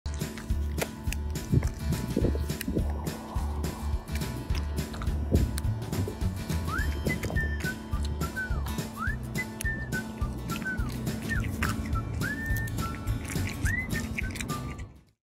Cute Corgi Snacks On Dandelions!
Enjoying your salad for lunch like... 🐶💕🥗